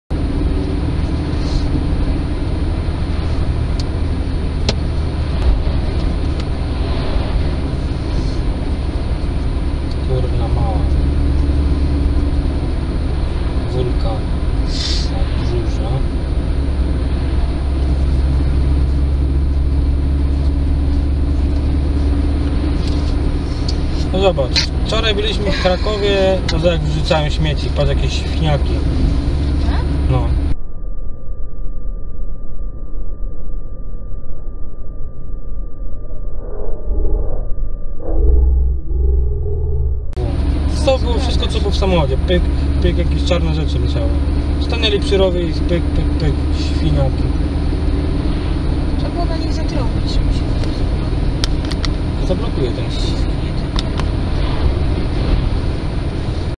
Obcokrajowcy opróżniają samochód ze śmieci rejestracja JDO 693 Turna Mała
Nagranie z dnia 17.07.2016. Widać jak jacyś obcokrajowcy wysypują kupę śmieci z samochodu o numerze rejestracyjnym JDO 693. Miejscowość Turna Mała.